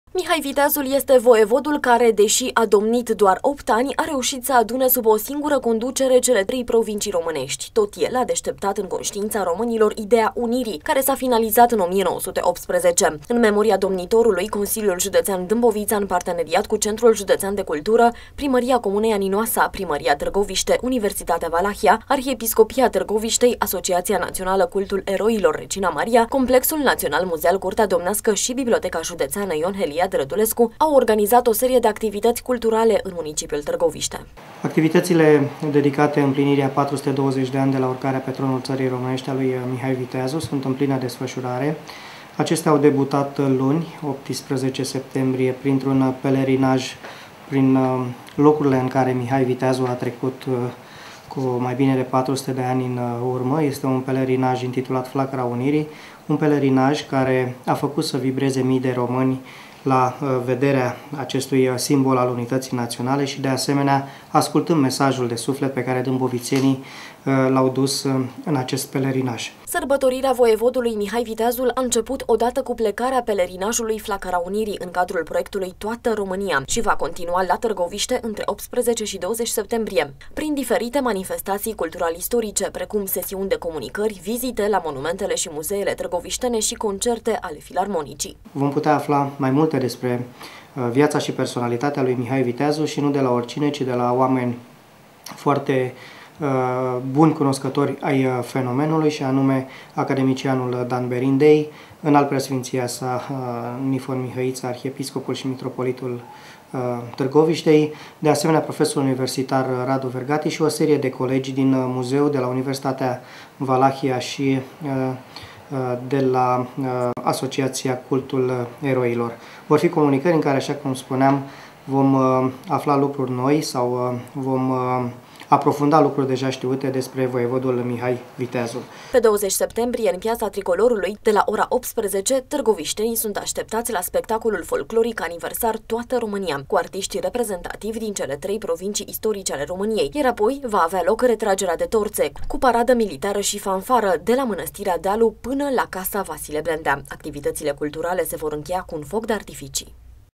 MIHAI VITEAZUL SĂRBĂTORIT LA TÂRGOVIȘTE (Columna Tv)
In perioada 18-20 septembrie 2013, pe raza municipiului Targoviste se vor desfasura mai multe manifestatii cultural-istorice, cu ocazia implinirii a 420 de ani de la urcarea pe tronul Tarii Romanesti a domnitorului Mihai Viteazul. Acestea vor fi organizate de Consiliul Judetean Dambovita.